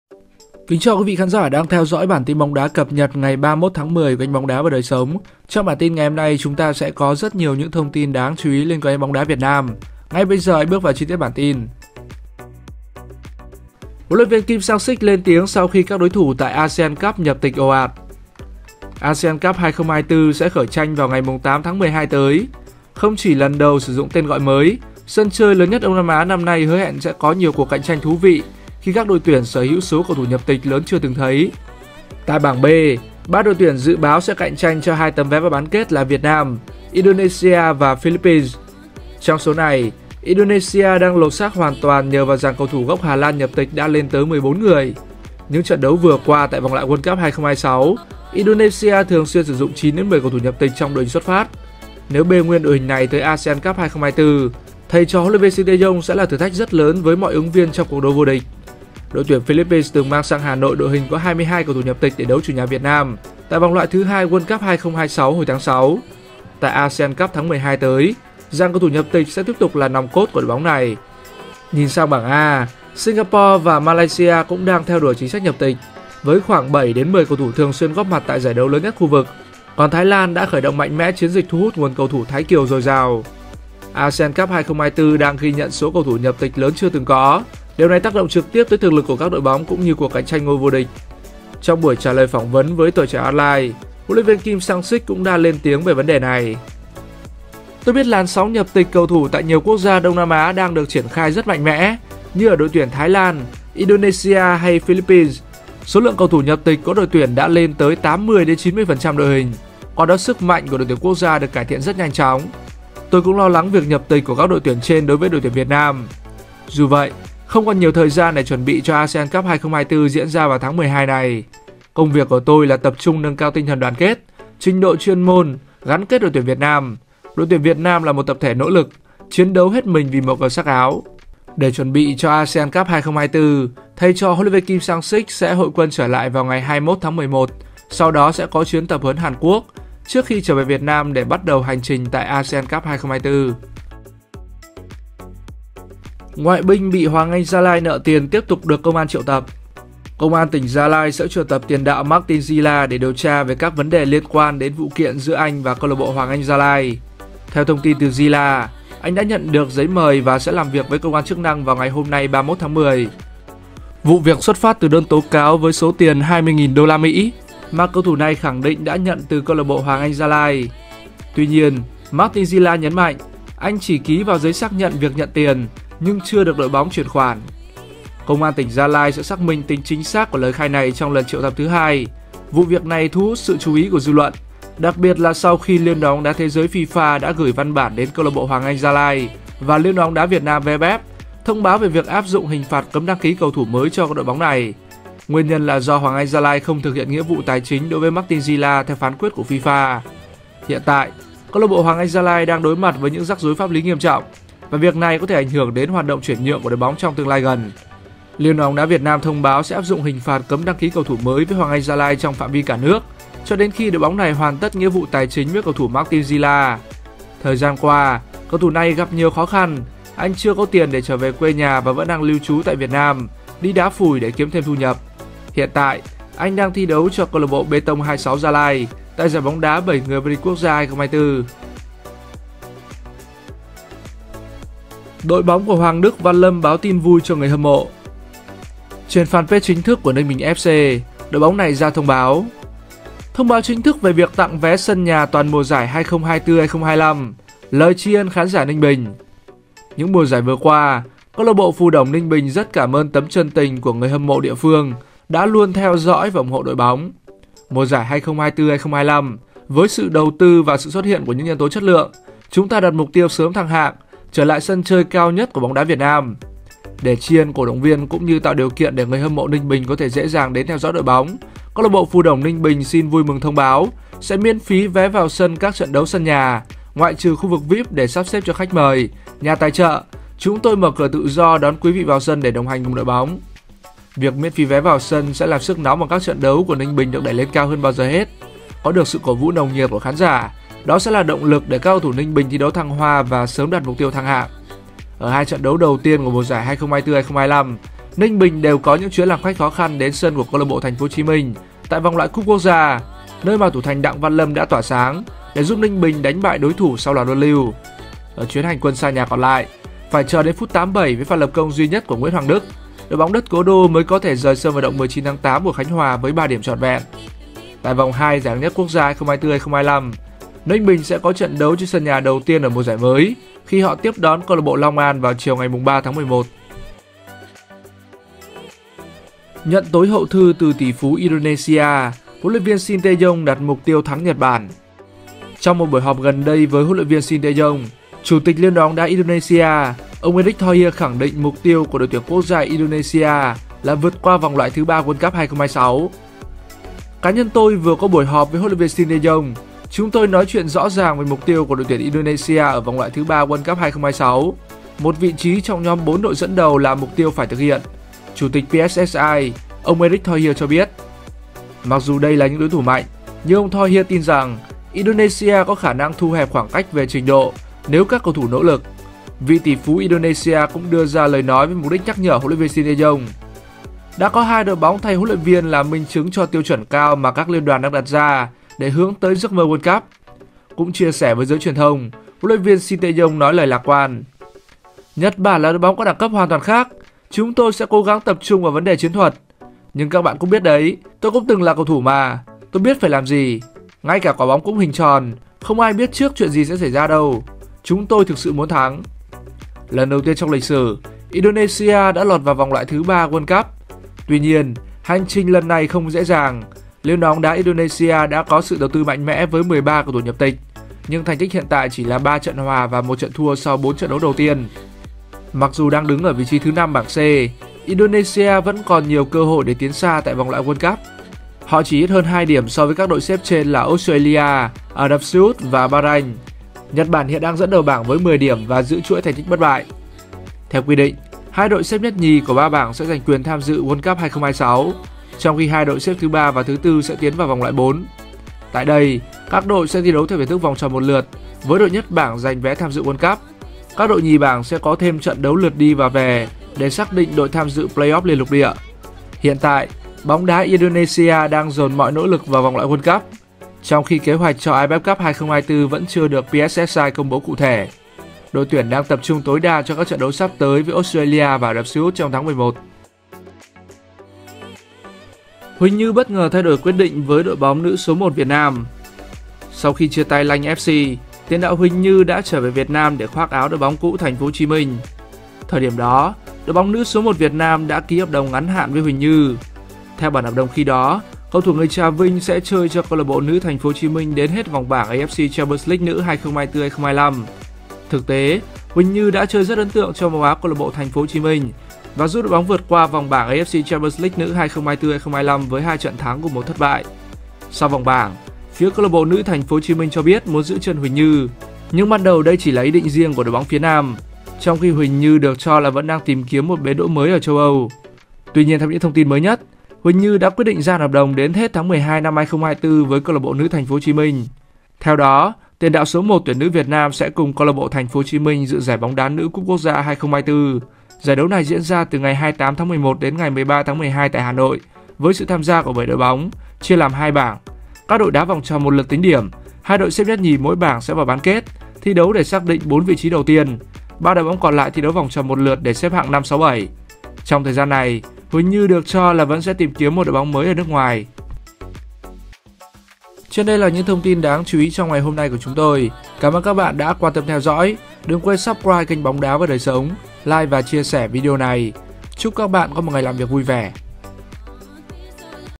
Tin HOT BĐ 31/10: Cựu sao HAGL bị công an triệu tập | HLV Kim Sang Sik lên tiếng về việc nhập tịch
0:00 Điểm tin
0:13 HLV Kim Sang Sik lên tiếng về chuyện nhập tịch
2:25 Cựu HAGL bị công an triệu tập
3:54 Đội bóng của Hoàng Đức, Văn Lâm báo tin vui tới NHM
5:28 HLV Shin Tae Yong đặt mục tiêu cực cao
7:45 Tiền đạo Huỳnh Như chốt tương lai